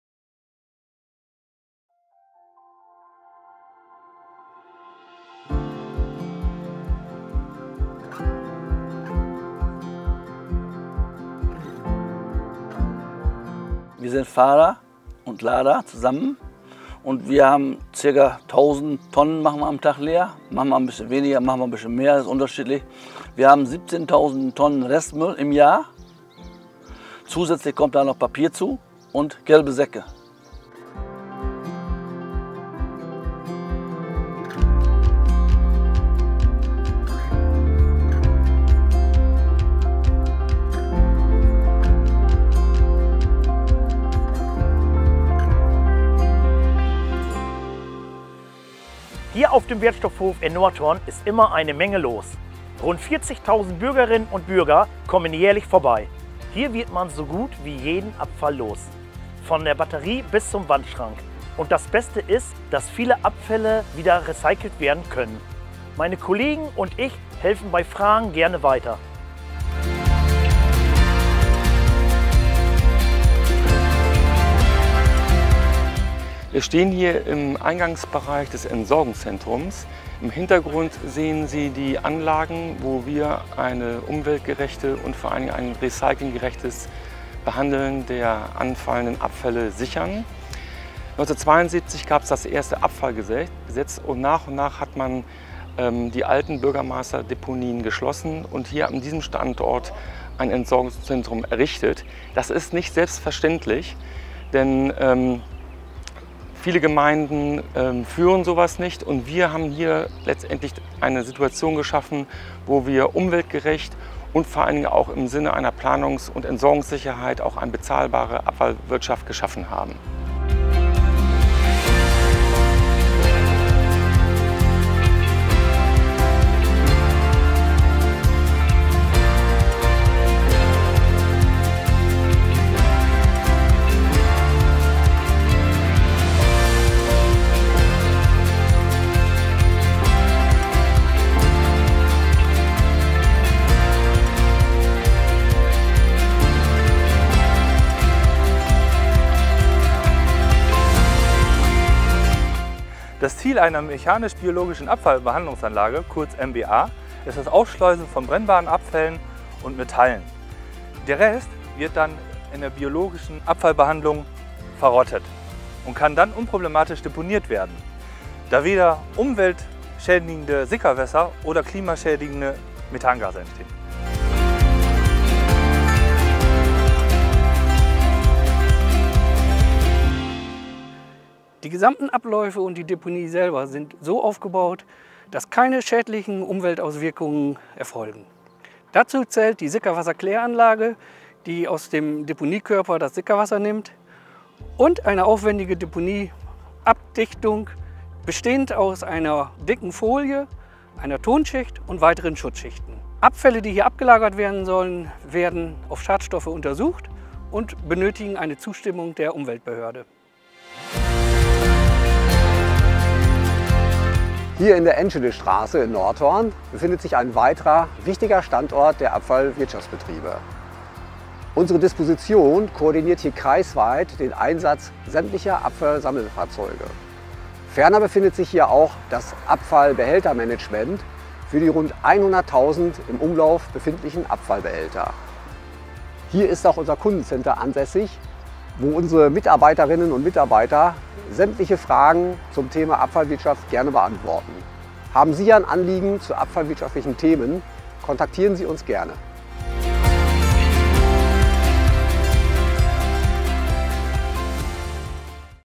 Vorstellung des Abfallwirtschaftsbetrieb Grafschaft Bentheim (AWB)
Was passiert mit den Abfällen im Landkreis Grafschaft Bentheim? Der AWB nimmt uns mit zu seinen Standorten im Landkreis Grafschaft Bentheim und stellt sein Leistungsangebot vor.